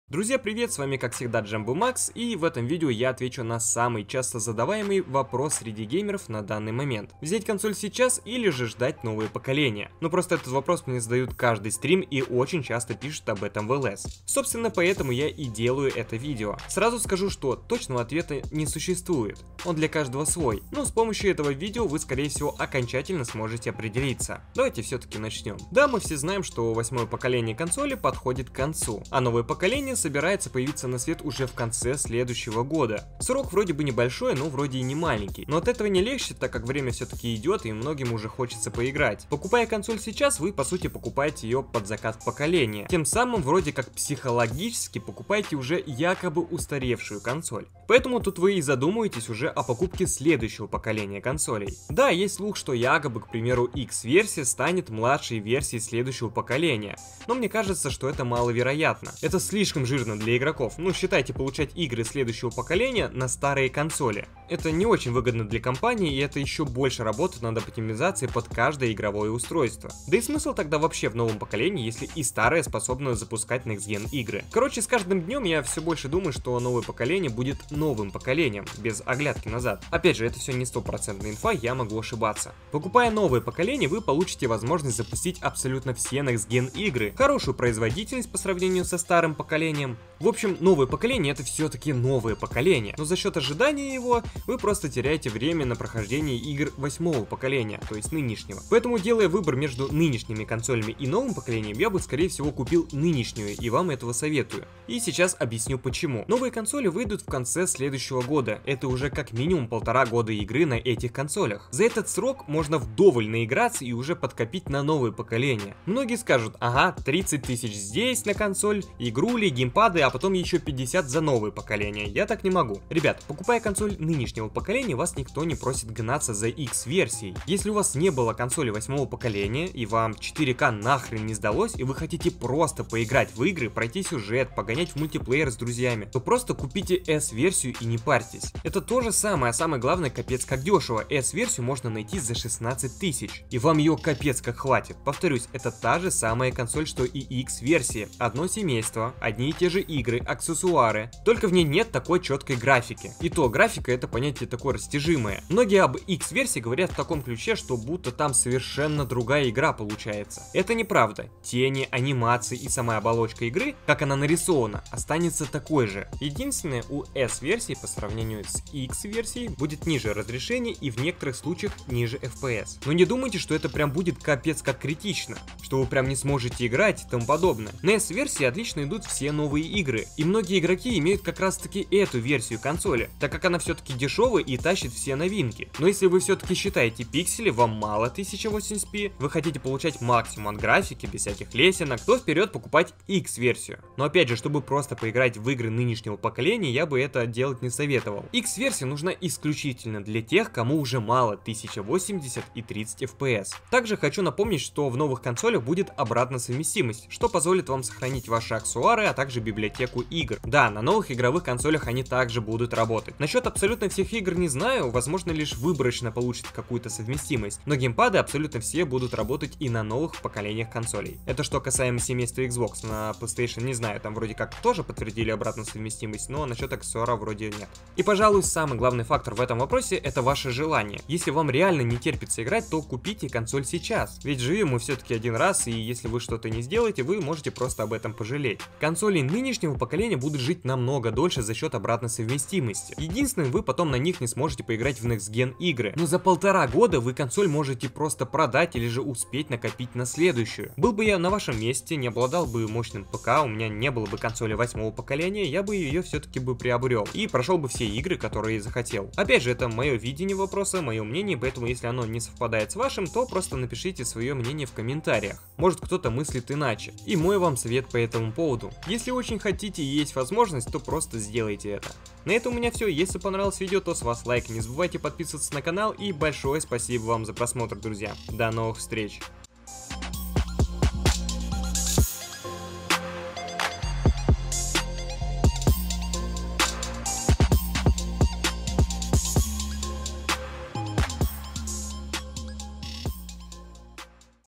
Купить Xbox One или Xbox Scarlett ? | 8-ое или 9-ое поколение | ДЕЛАЕМ ВЫБОР
В этом видео вы узнаете стоит ли покупать новое поколение консолей на данный момент.